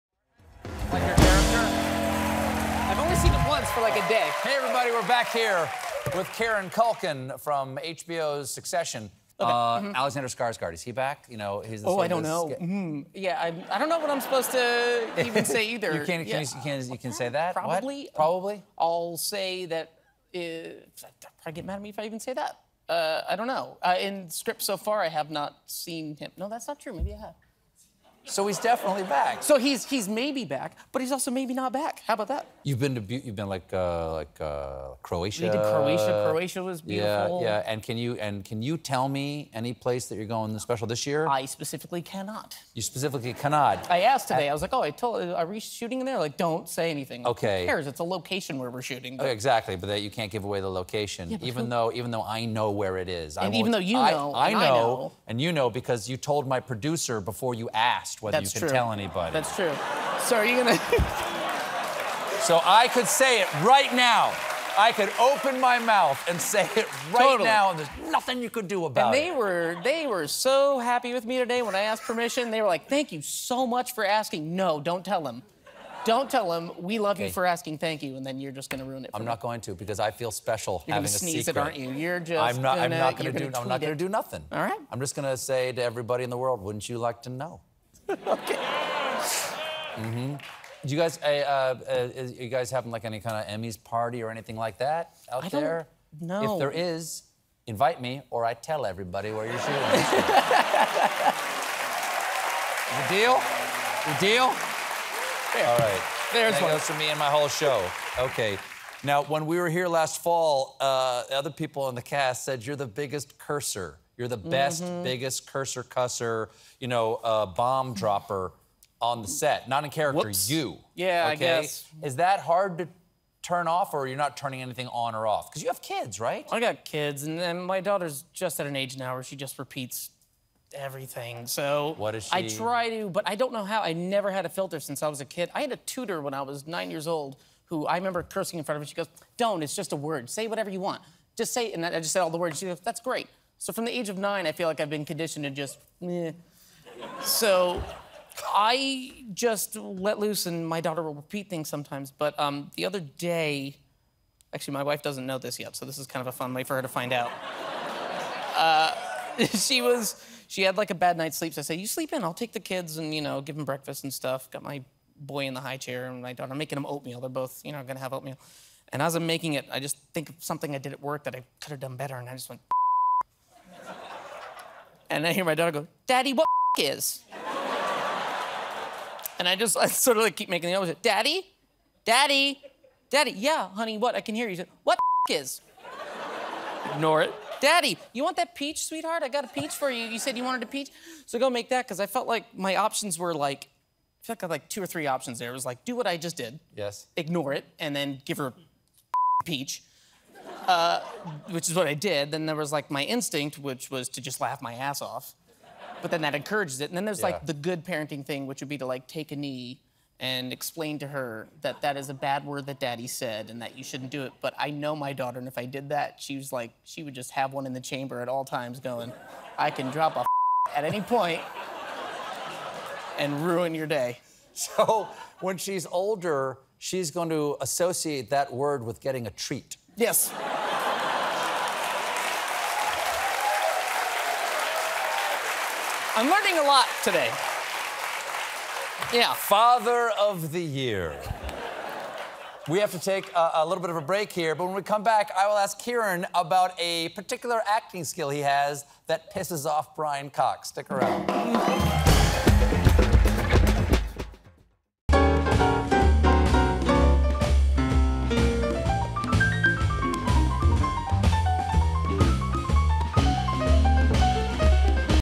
Father Of The Year Kieran Culkin On The Perils Of Cursing In Front Of His Kids
'Succession' star and well-known potty mouth Kieran Culkin confesses to dropping some curse words in front of his young children, and predicts he'll soon pay a price for his slip up. Stick around for more with the Emmy-nominated Kieran Culkin!